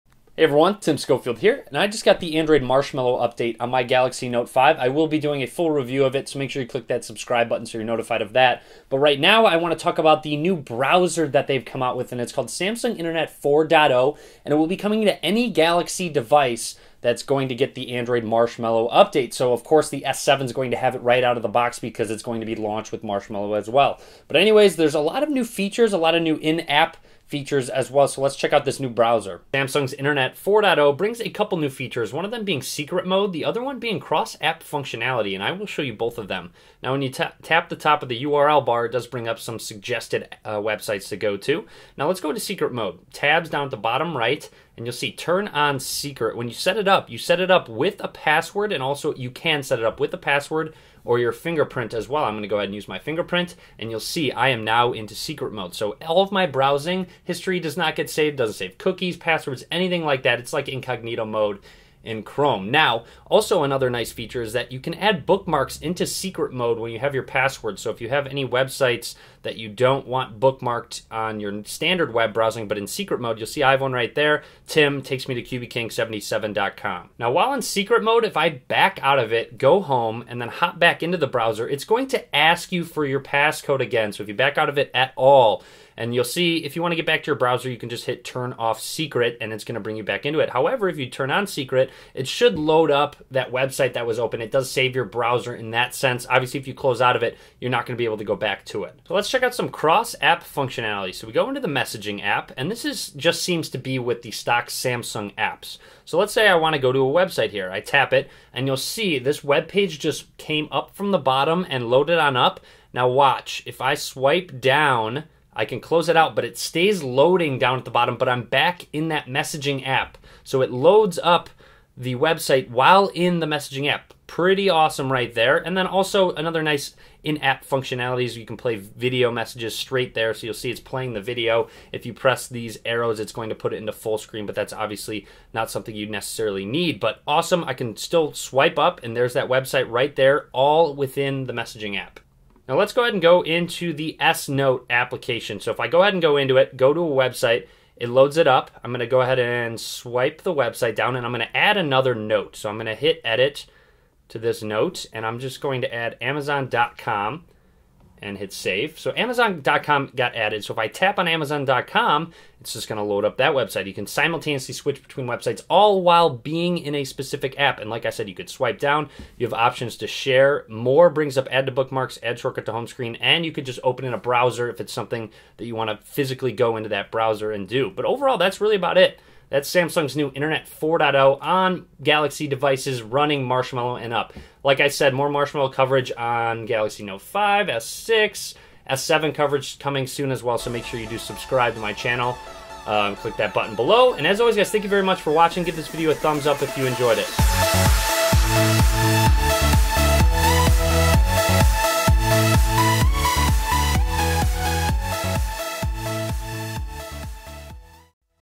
Samsung Internet 4.0 on Galaxy Devices with Android 6.0 Marshmallow
Here is the new browser that will be added to ALL Samsung Galaxy devices that get updated to Android 6.0 Marshmallow. This browser adds a new privacy mode and also an awesome cross app feature.

The Galaxy S7 is launching with Marshmallow, but this will come to the S6 as well and any other Galaxy device updating to Android 6.0.